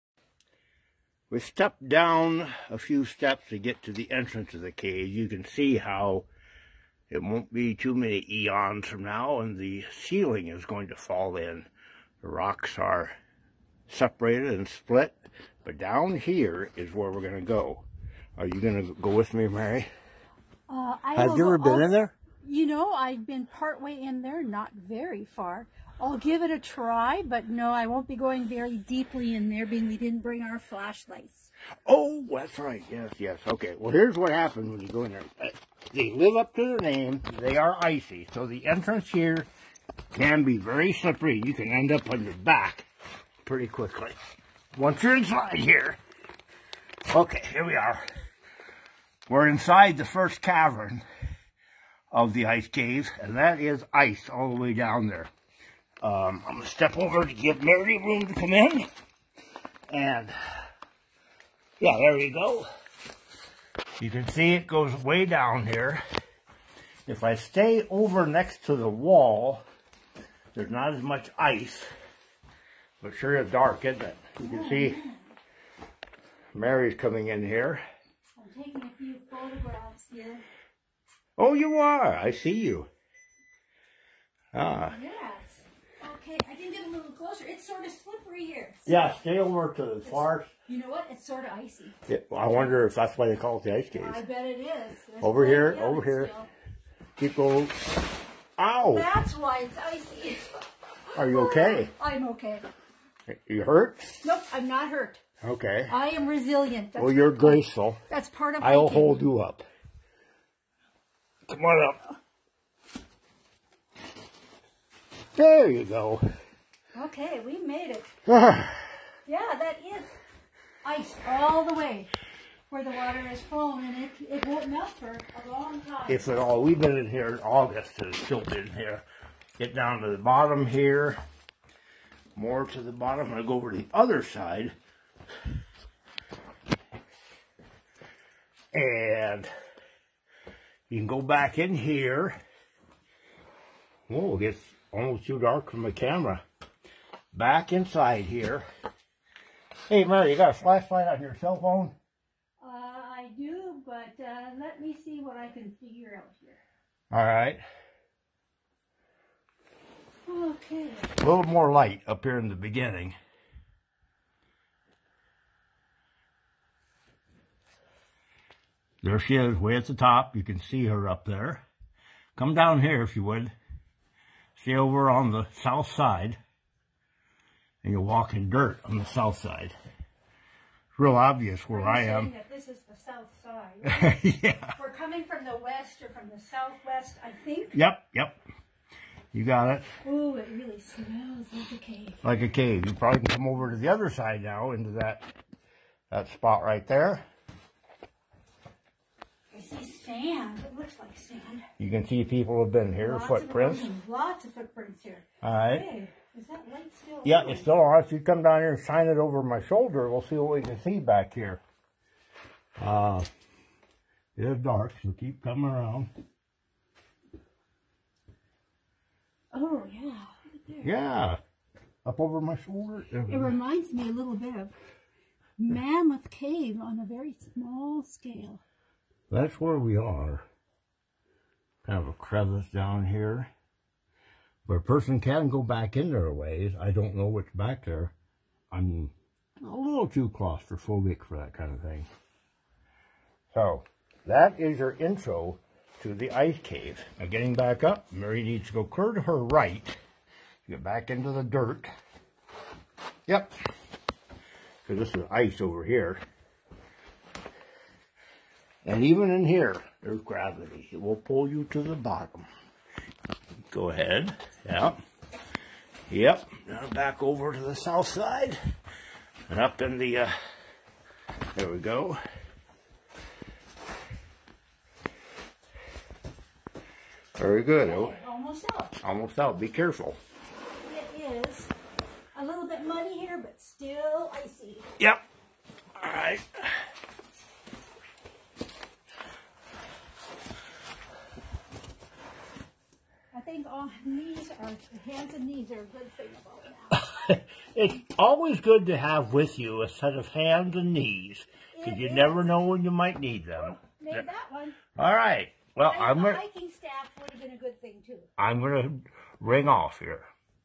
Ice Caves in the Rugged Badlands of North Dakota? Yes!!
Two hikers find and climb down in to one of North Dakota's caves. It was first a long drive into the interior of the North Dakota Badlands, then a short hike and then in to the dark Ice Caves.